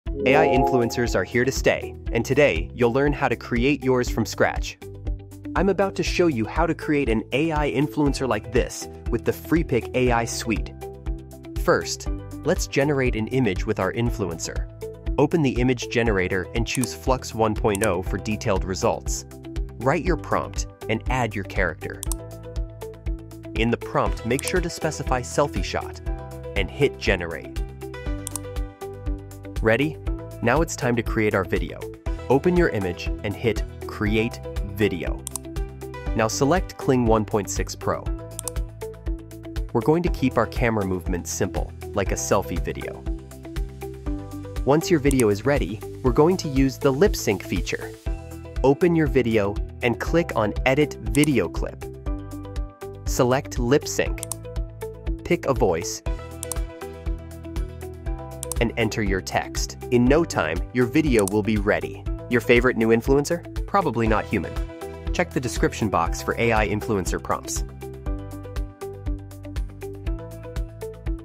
How to make AI influencers that look real | Freepik How-To
Thinking about building a digital personality? This AI influencer tutorial walks you through how to create an AI influencer from scratch. From designing the look to posting content, it’s all broken down in a simple, clear way. No special skills needed.